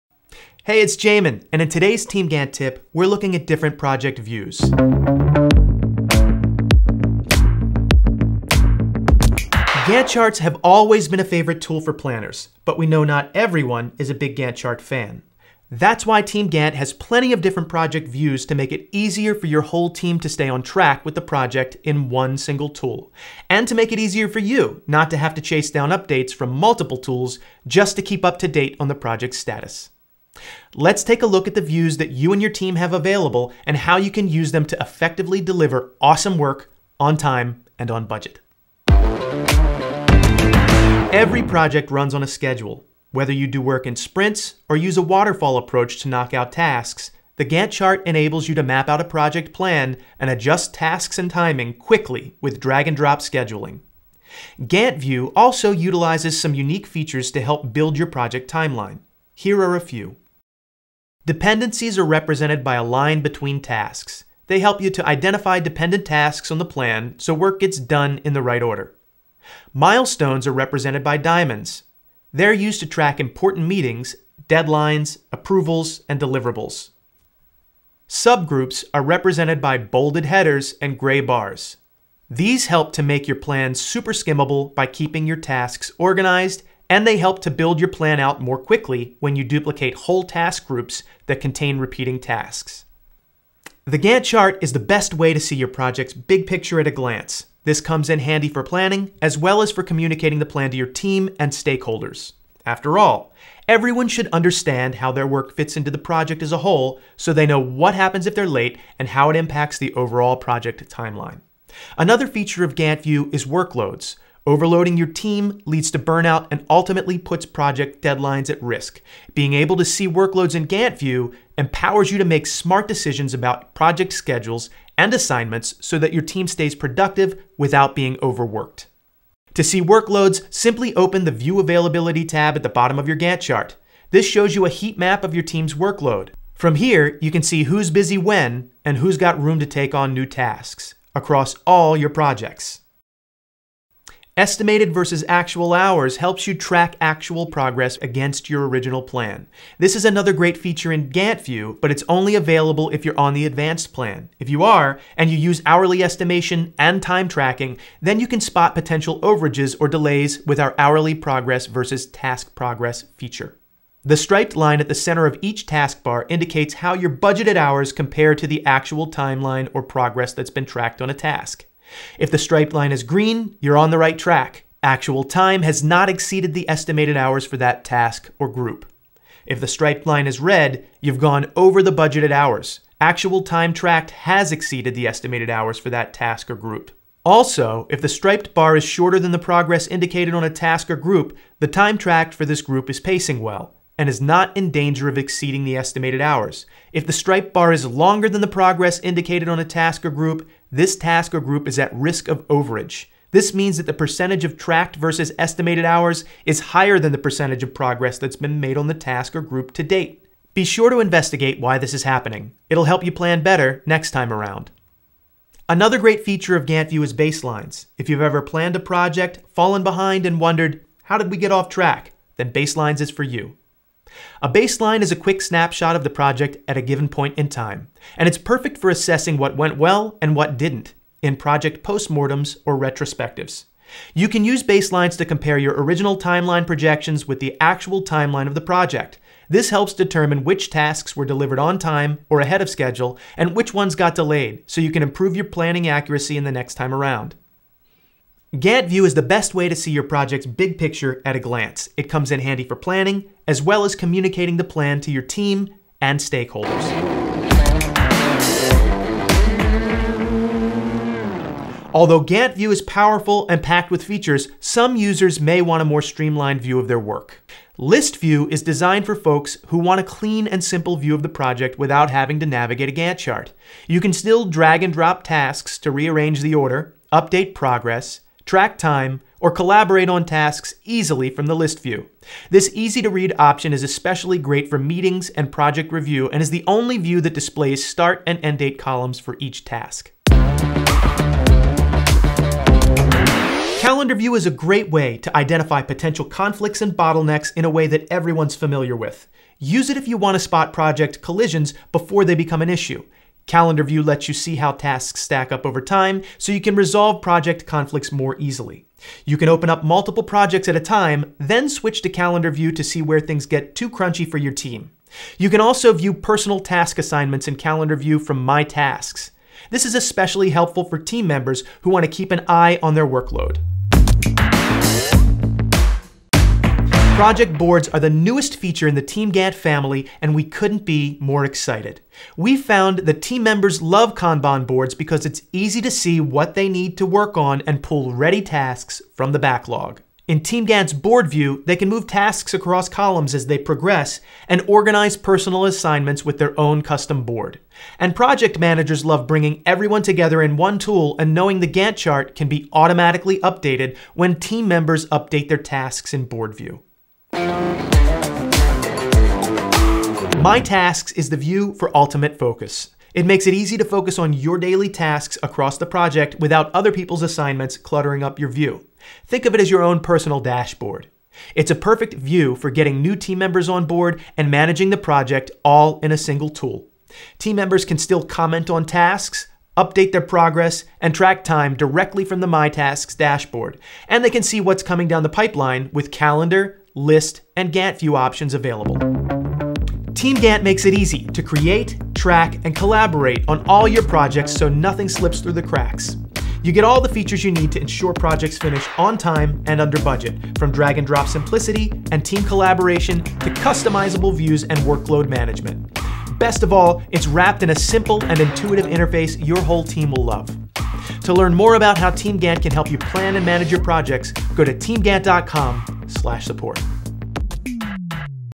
How to Use Different Project Views | TeamGantt Tips
Learn how to use different project views in TeamGantt, so you and your team can find a workflow that works best for every project situation—without having to juggle multiple project management tools. Try TeamGantt for free

Gantt charts have always been a favorite tool for planners, but we know not everyone is a big gantt chart fan.

You shouldn’t have to chase down updates from multiple tools just to keep up-to-date on project status. That’s why TeamGantt has plenty of different project views to make it easier for your whole team to stay on track with the project in one single tool.

In this video, we’ll show you all the views TeamGantt has available and how you can use them to effectively deliver awesome work on time and on budget.

- Gantt chart view
- Task list view
- Calendar view
- Kanban board view
- My Tasks view

For a closer look at how to use different TeamGantt views, check out this blog post: